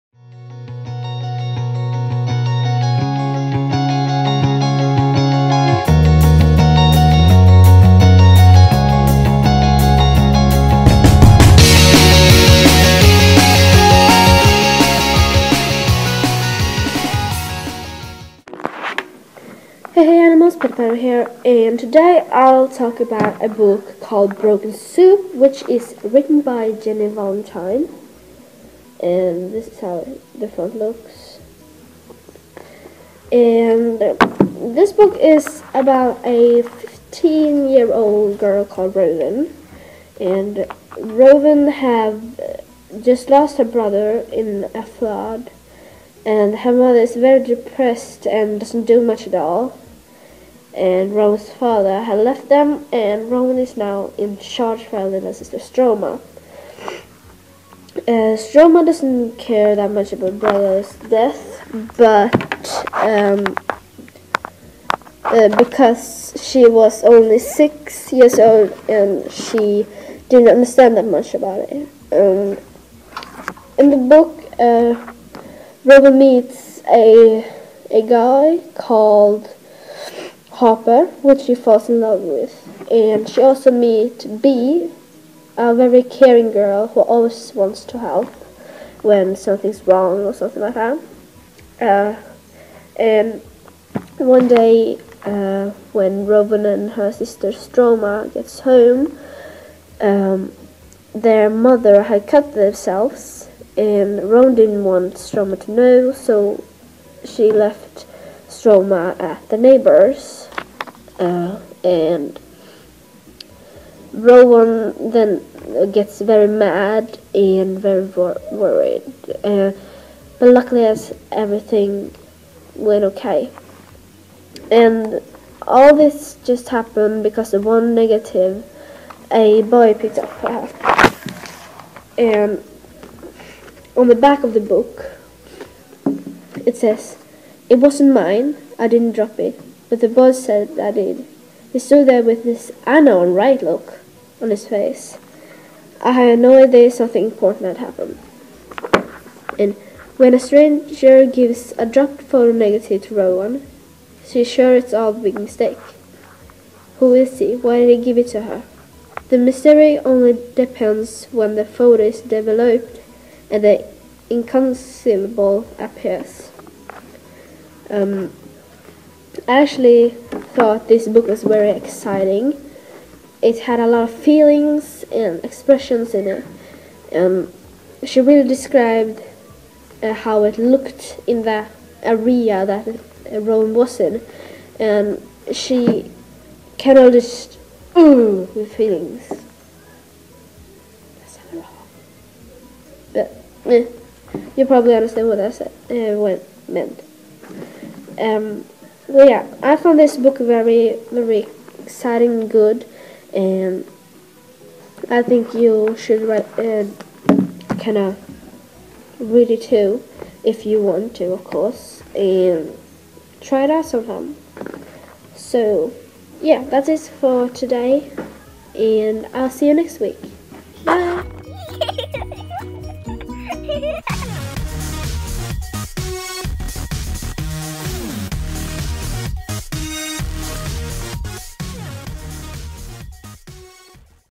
Book Review Ep 1 {¶} Broken Soup
Broken soup is a perfect book for you who like romance and tragedy. Hope you have a great day! ♥️♥️